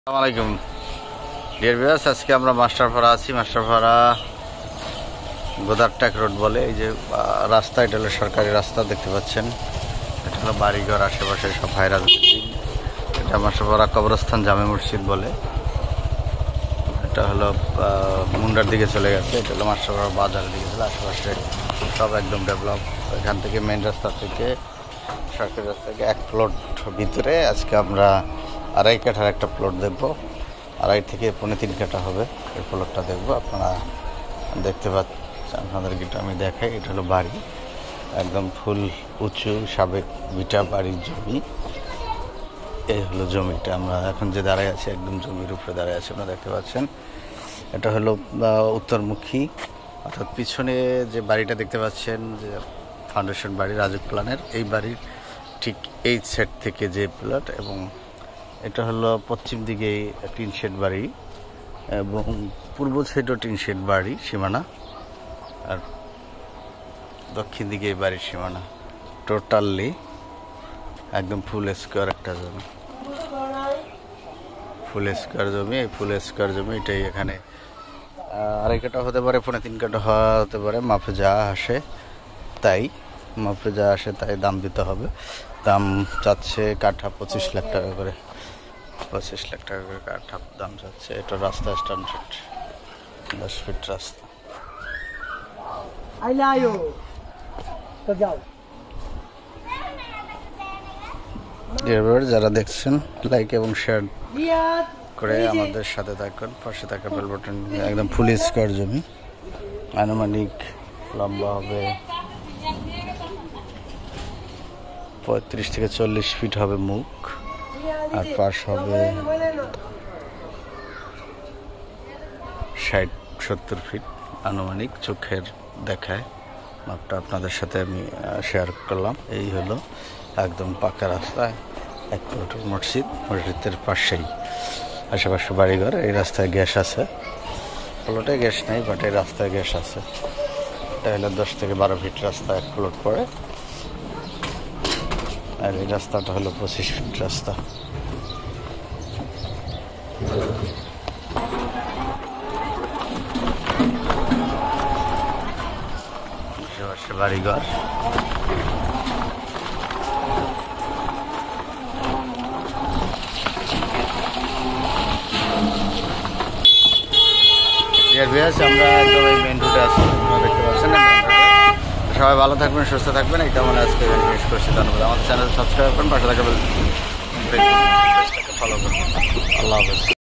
landforsale 2.75 Katha plot in master para uttarkhan dhaka uttara homecareproperty#01955560500
price per katha 25 lac tk

location : মাষ্টারপাড়া বাজার গোদারটেক রোডের পাশে উত্তরখান ঢাকা উত্তরা।

উত্তর আব্দুল্লাহপুর থেকে সামান্য দূরত্বে রেল গেটের পরে আটি পাড়া পর মাস্টার পাড়া বাজার কাছে ২.৫-২.৭৫ কাটা জমি বিক্রি স্কার শেপ জমি।

রাস্তা মেন রোড থেকে এক লোড ভেতরে ১০ ফিট রোডের সাথে

বিস্তারিত জানতে অভিজিৎ করতে একদিন আগে কল করুন।